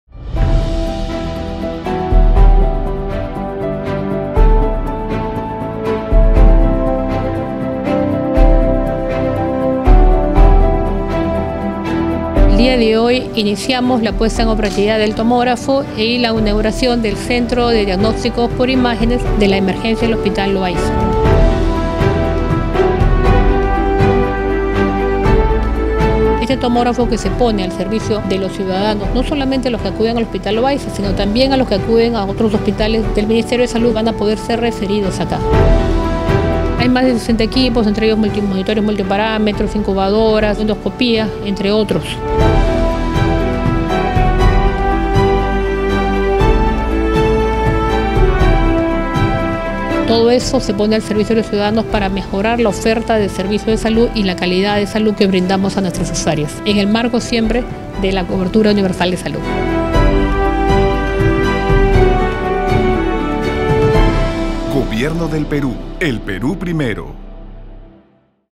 Entrega de tomógrafo y modernos equipos al Hospital Loayza
TuSaludPrimero 👉 Un moderno tomógrafo y más de 60 equipos médicos especializados fueron entregados al Hospital Nacional Arzobispo Loayza, un hito que marca el inicio de la mejora en la calidad de servicios para los pacientes de este emblemático nosocomio.